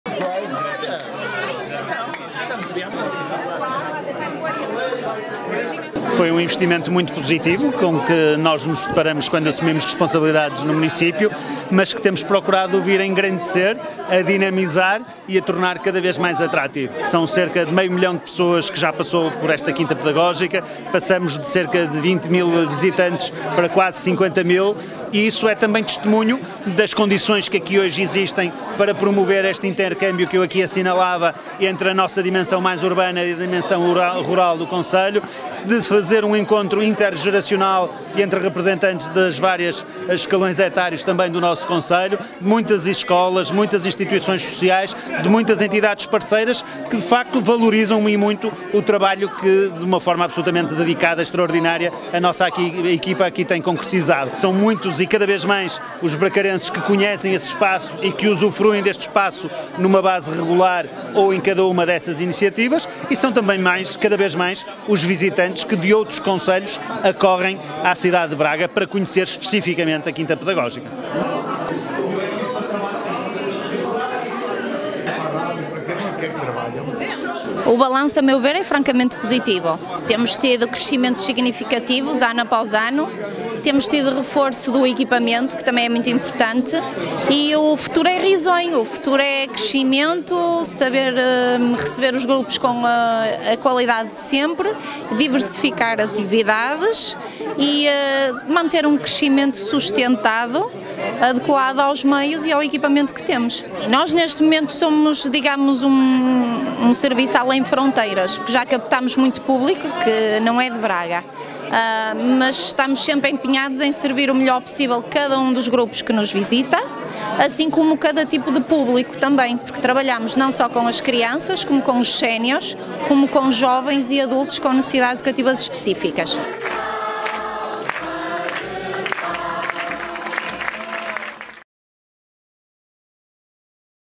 Quinta Pedagógica celebra 20 anos com mais de meio milhão de visitantes
A Quinta Pedagógica de Braga completou esta segunda-feira 20 anos e aproveitou para celebrar o marco de mais de meio milhão de visitantes que atingiu neste período, para o qual contou com o «número mágico» de 50 mil visitantes em 2023.